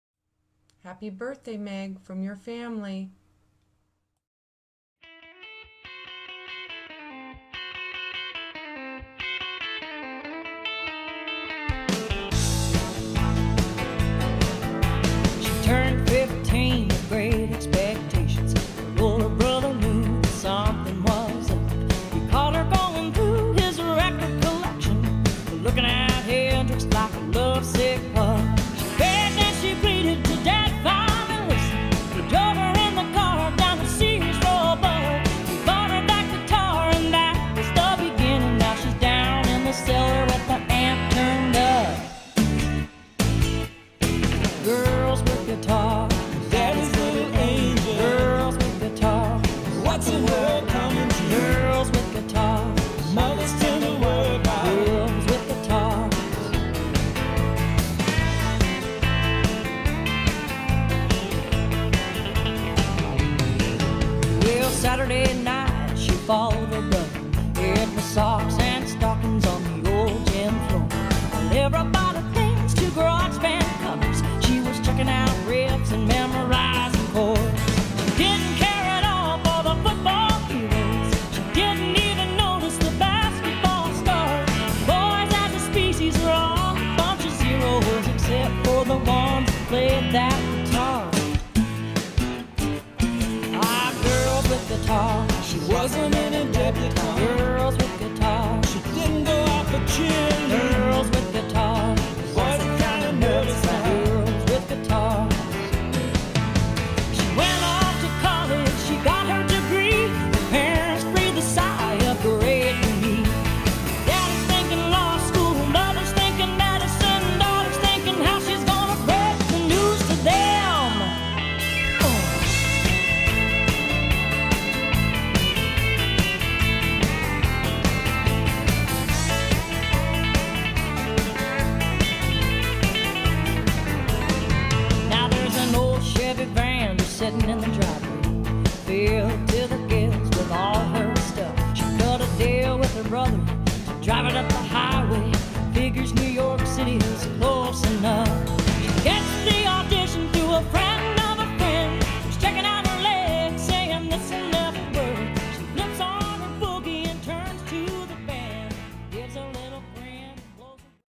Happy Birthday Meg 24 Years!!
This isn't much, but we sure hope you have a great day, and know how very much we love you. PS I sort of have videos on the brain, and it was the easiest way to send this birthday wish.
Love,  Mom, Dad, Jake & Sam